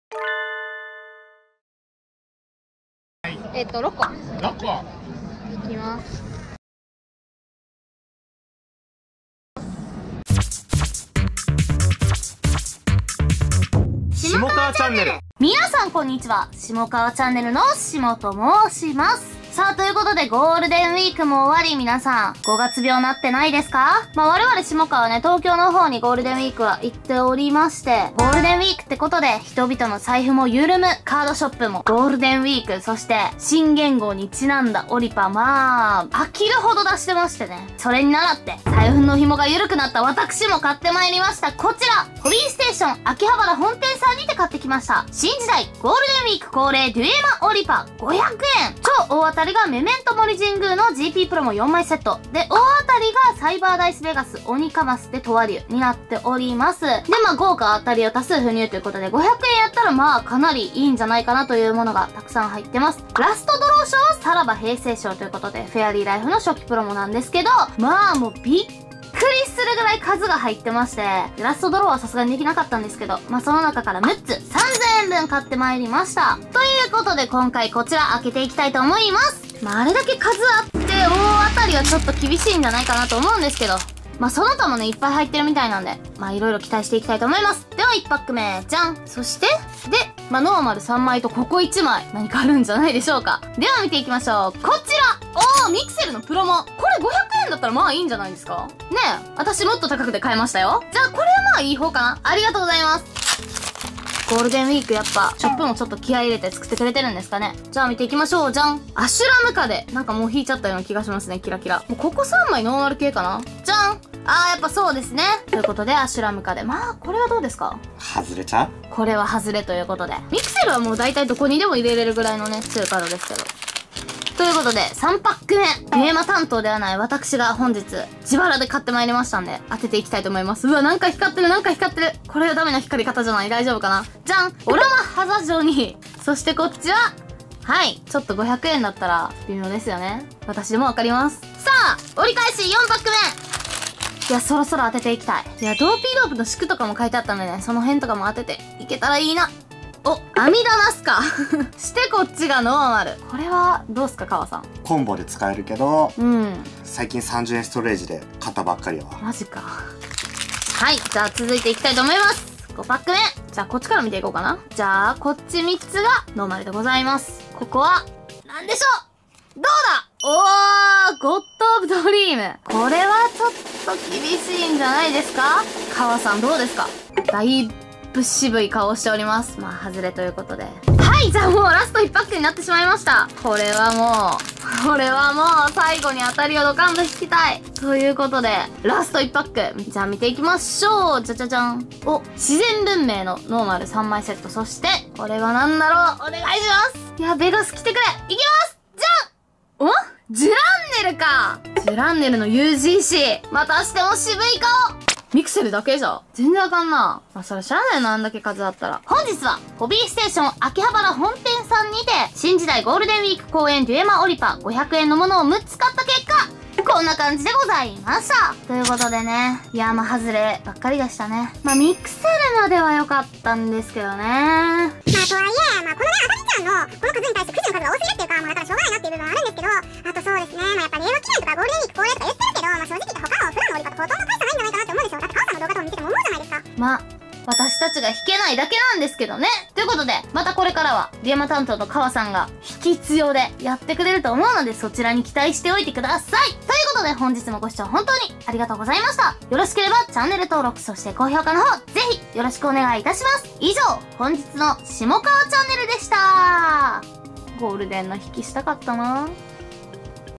【デュエマ】"引き強担当"の『私』に、デュエマくじは任しといて下さい。 ～GW限定！東京オリパ～【開封動画】
デュエルマスターズ500円オリパ6口分の開封動画です。
(東京・ホビーステーション秋葉原本店)
さぁ、さぁ、さぁ！！！

・今日のオススメ動画
カードショップ激戦区で『1万円福袋』を購入した20代・男性(自称YouTuber)

シモカワ公式@shimokawa_ch
シモ@shimo_erishita
カワ@kawa_DMyoutube